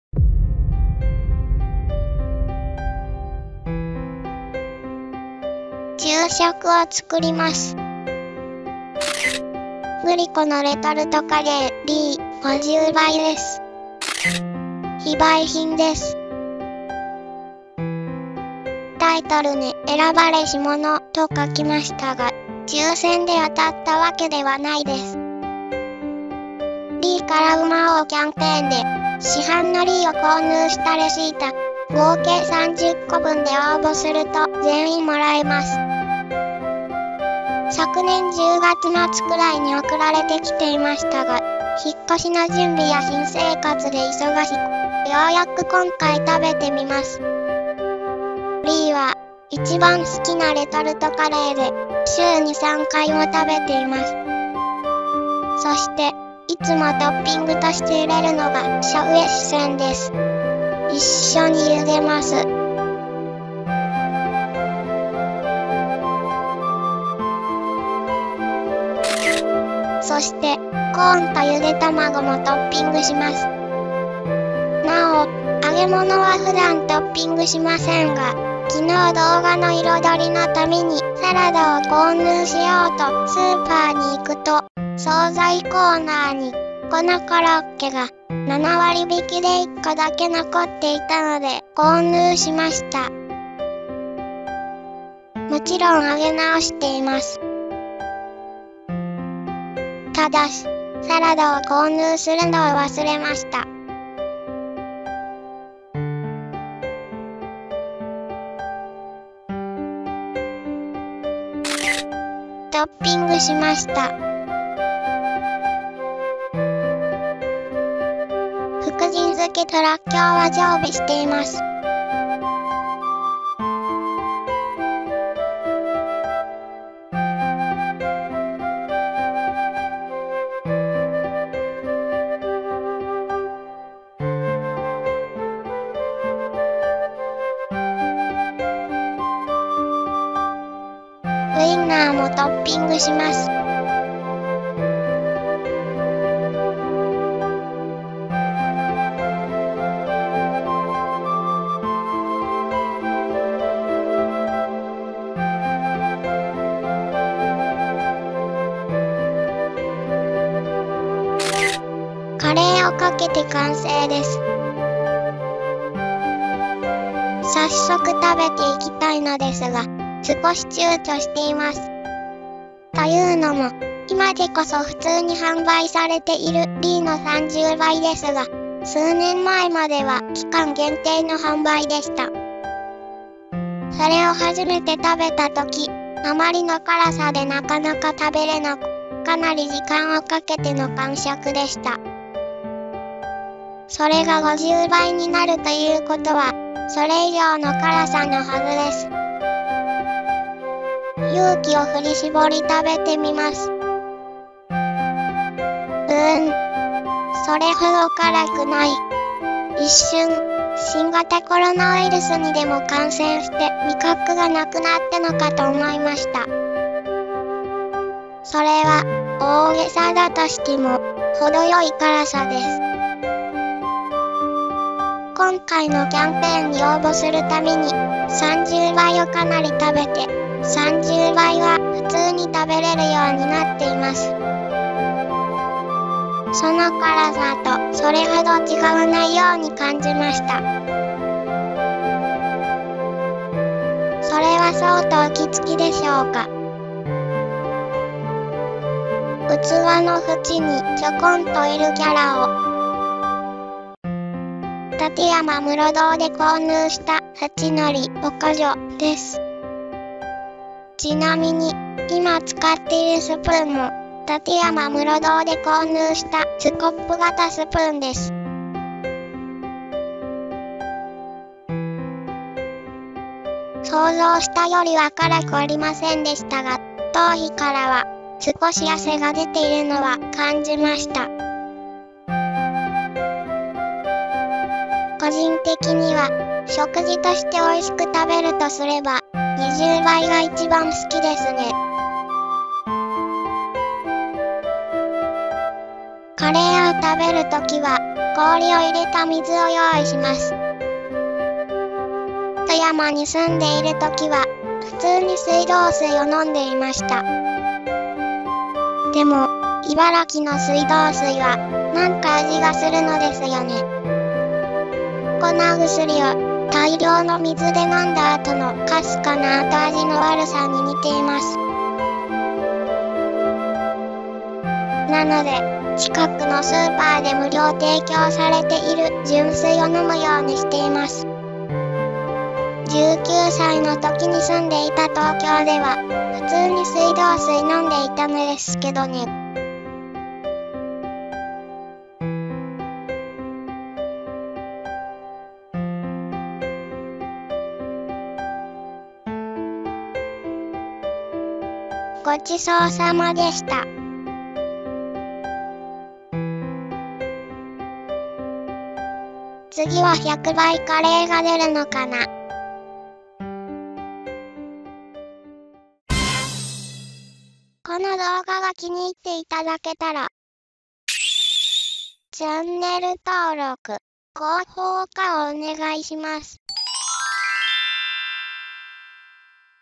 《選ばれし者のみが食べることを許されたカレー》グリコレトルトカレー LEE 50倍（20230312撮影）
通常のLEE　３０箱購入するともらえる『LEE 50倍レトルトカレー』を食べてみました。その辛さは・・・